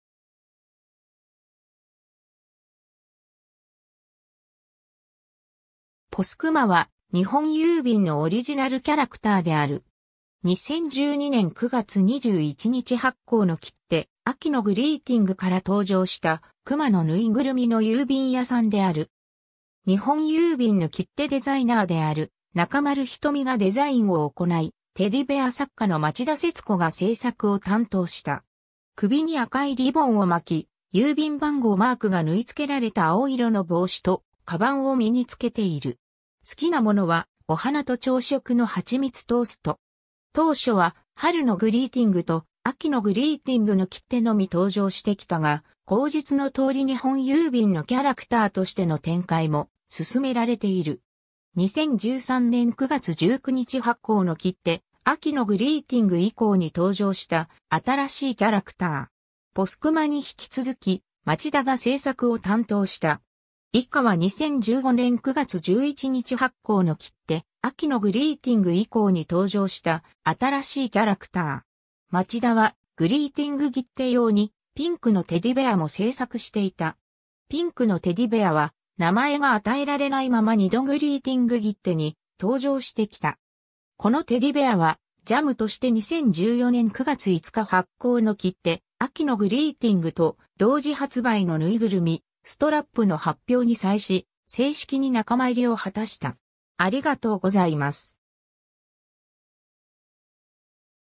ぽすくま
ぽすくまは、日本郵便のオリジナルキャラクターである。
 2012年9月21日発行の切手「秋のグリーティング」から登場した、クマのぬいぐるみの郵便屋さんである。
日本郵便の切手デザイナーである中丸ひとみがデザインを行い、テディベア作家の町田節子が制作を担当した。
 首に赤いリボンを巻き、〒マークが縫い付けられた青色の帽子とカバンを身につけている。
好きなものはお花と朝食のはちみつトースト。
 当初は「春のグリーティング」と「秋のグリーティング」の切手のみ登場してきたが、後述の通り日本郵便のキャラクターとしての展開も進められている。
 2013年9月19日発行の切手「秋のグリーティング」以降に登場した、新しいキャラクター。
ぽすくまに引き続き町田が制作を担当した。
 以下は2015年9月11日発行の切手「秋のグリーティング」以降に登場した、新しいキャラクター。
 町田はグリーティング切手用にピンクのテディベアも制作していた。
ピンクのテディベアは名前が与えられないまま2度グリーティング切手に登場してきた。
 このテディベアは「じゃむ」として2014年9月5日発行の切手「秋のグリーティング」と同時発売のぬいぐるみ・ストラップの発表に際し、正式に仲間入りを果たした。